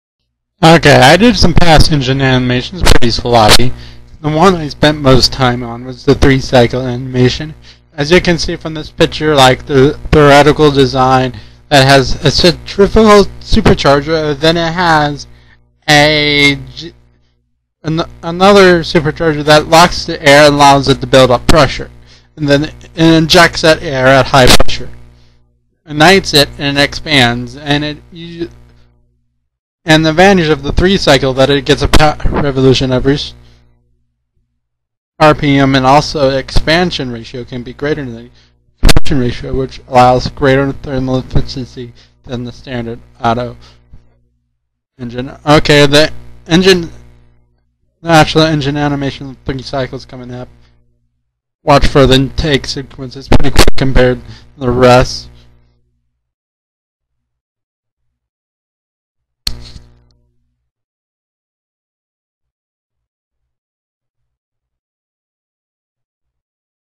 three cycle engine explantion
I show old engine anamitions. I then very brfiely tell how three cycle engine works